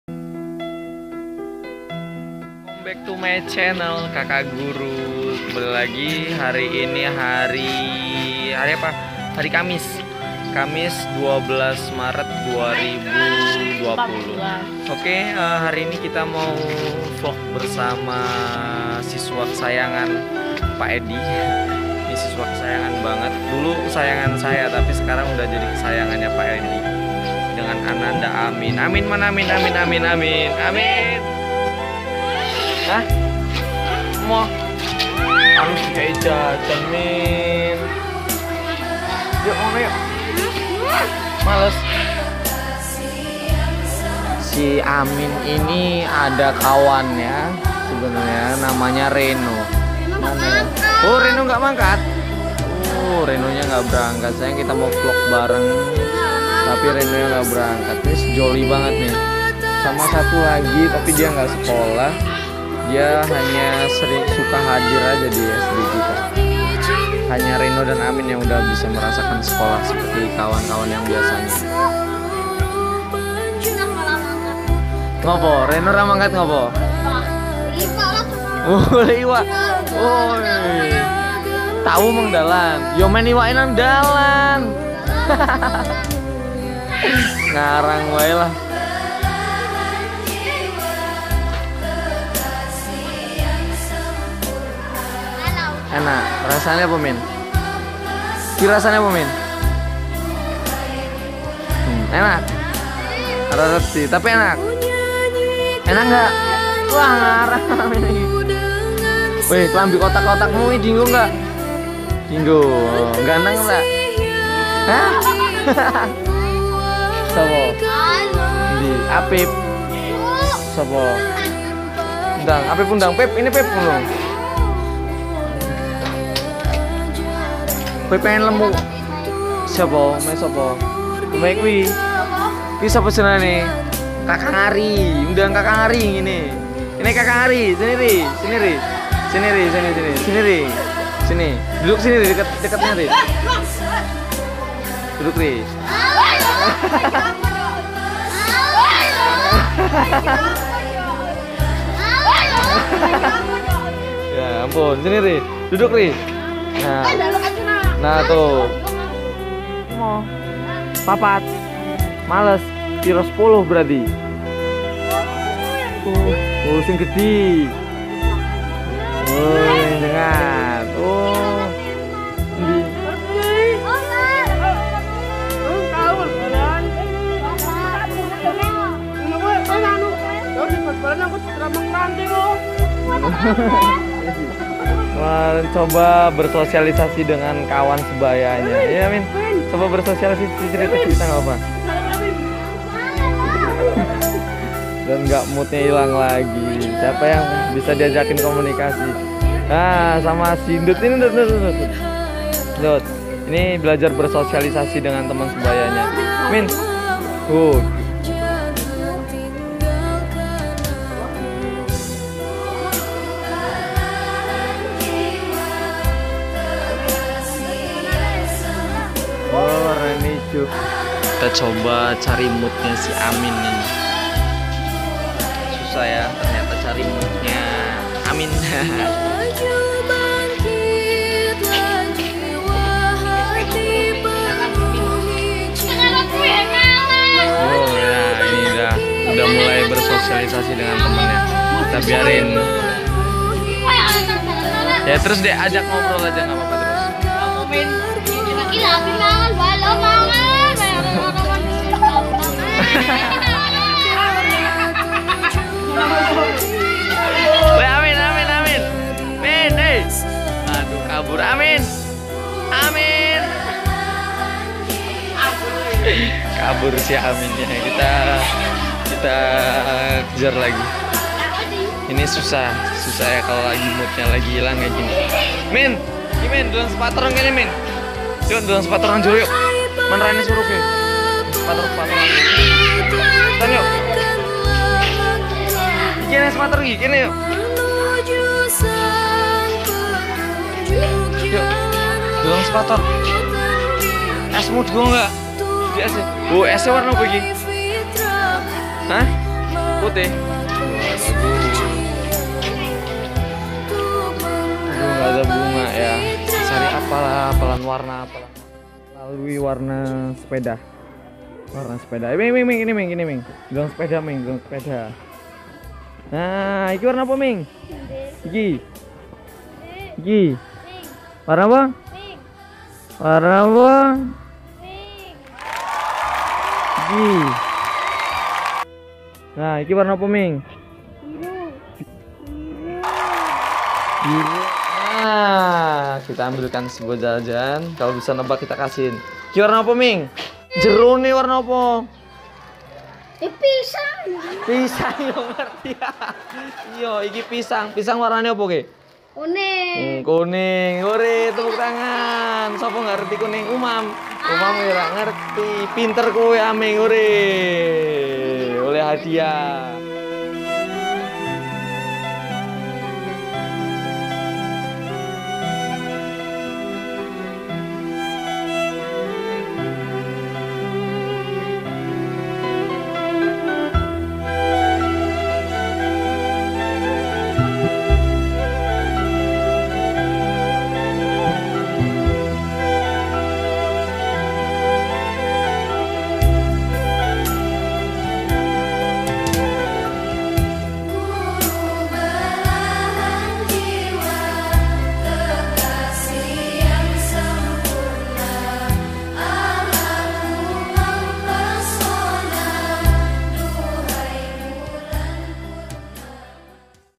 AMIN CURHAT, BIKIN NANGIS
Pemblajaran kepada salah satu siswa istimewa (berkebutuhan khusus) di waktu istirahat. Pembelajaran di lakukan untuk mengasah kemampuan bersosialisasi sekaligus mengulang pelajaran - pelajaran yg telah lampau di pelajari.
Semoga bermanfaat dan berguna.
Terimakasih